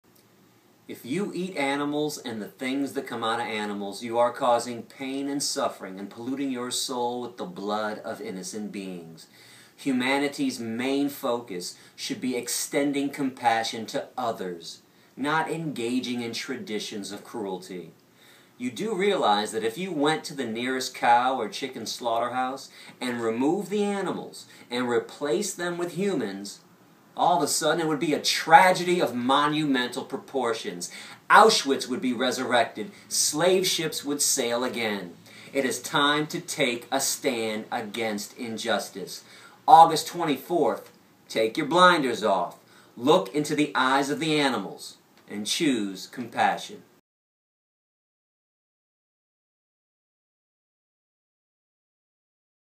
תסתכלו להם בעיניים: גארי יורופסקי מזמין אתכם להצטרף לצעדת הענק (תרגום לעברית)
במוצ"ש 24.8.13 ב-20:00 נתאסף בת"א, שד' רוטשילד 31, כל הדואגים לזכויות בעלי החיים. נצא בתהלוכה ברישיון המשטרה ובשמירתה ונצעד להבימה ומשם לסינמטק. נקרא להפסקת ניצול בעלי החיים: די לשקרים ודי להסתרה. נדרוש את סיום האלימות כלפי בעלי החיים: בתעשיות המזון מן החי; בתעשיית הניסויים; בתעשיית הפרווה והעור; בתעשיית ההמתה של חיות הבית, כלבים וחתולים; בתעשיית הבידור ובמרחב החופשי (חיות הבר). נזעק את כאבן של החיות כולן ונשמיע את קולן באהבה ובחמלה. כל מי שחרד לזכויות בעלי-החיים וקורא להפסקת הניצול והאלימות כלפיהם - מוזמנים.